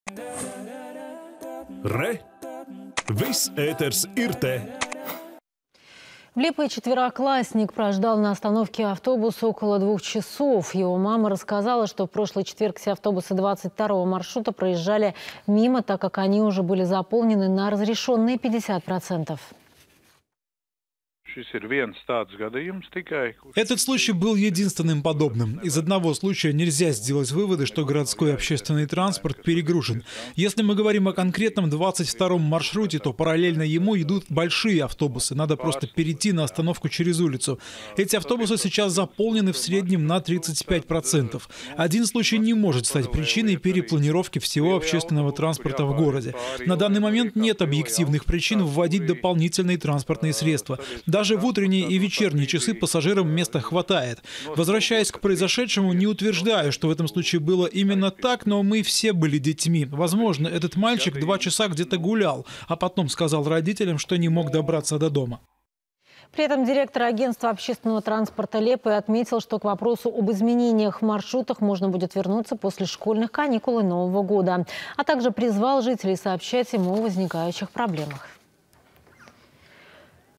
2 часа в ожидании автобуса
В Лиепае четвероклассник прождал на остановке автобуса около двух часов. Его мама рассказала, что в прошлый четверг все автобусы 22 маршрута проезжали мимо – т. к. они уже были заполнены на разрешенные 50%.